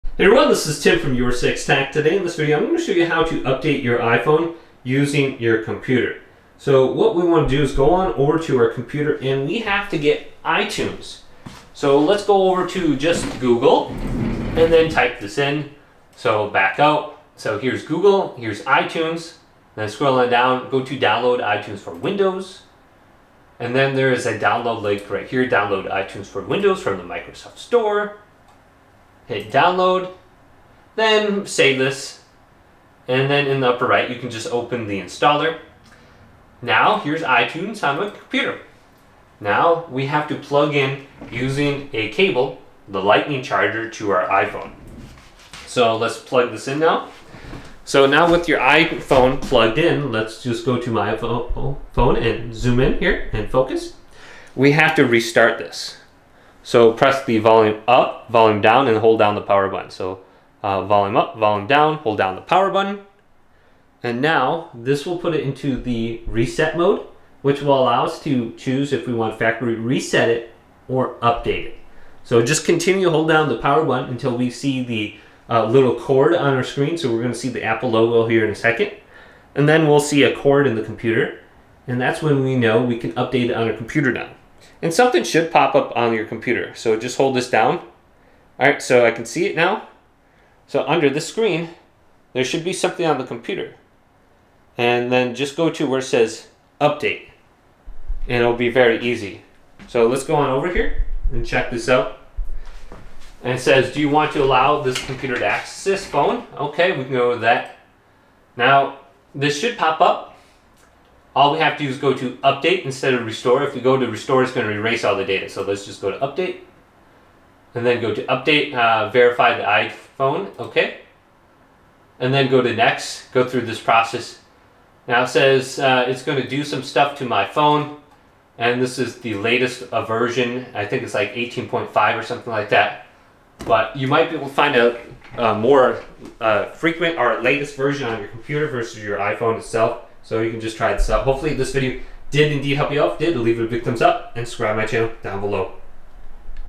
How to update your iPhone using your Computer (Easy Method)
Steps to Update iPhone Using a Computer
Step 1: Back Up Your iPhone (Optional but Recommended)
Connect your iPhone to the computer using the USB cable.
Open Finder or iTunes:

On macOS Catalina or later, open Finder and select your iPhone under “Locations” in the sidebar.
On Windows or macOS Mojave or earlier, open iTunes and click the iPhone icon in the top-left corner.

Create a Backup:

In Finder/iTunes, under “Backups,” select Back up to this Mac (Finder) or This computer (iTunes).

Check Encrypt local backup if you want to include passwords and health data (you’ll need to set a password).

Click Back Up Now and wait for the backup to complete.
Verify the backup in Finder/iTunes under the backup list or in Preferences - Devices (iTunes).